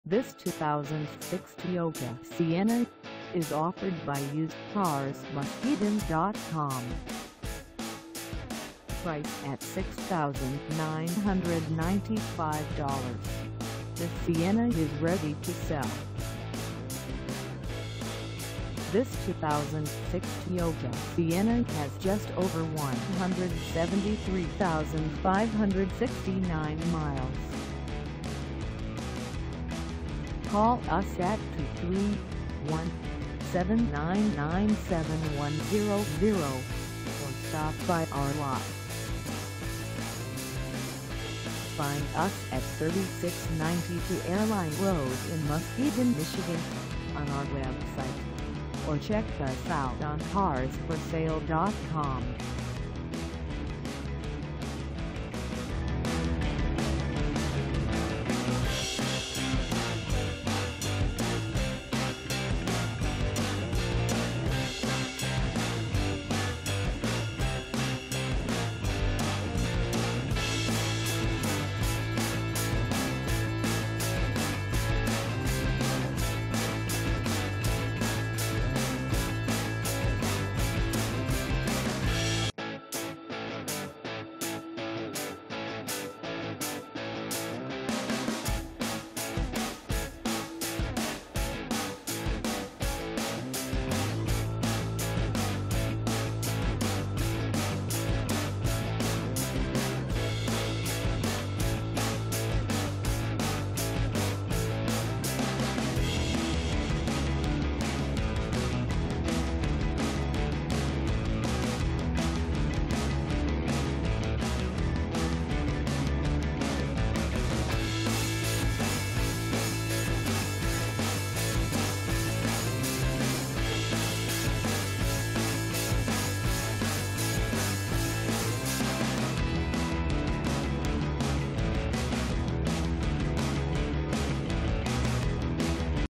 2006 Toyota Sienna XLE for sale in Muskegon, MI 49444 at Use
This 2006 Toyota Sienna XLE is for sale in Muskegon, MI 49444 at UsedcarsMuskegon.com.

Engine: 3.3L V6
Transmission: Automatic 5-Speed
Color: Desert Sand Mica
Mileage: 173569
Contact UsedcarsMuskegon.com 3692 Airline Rd. Muskegon, MI 49444 to test drive this 2006 Toyota Sienna XLE today.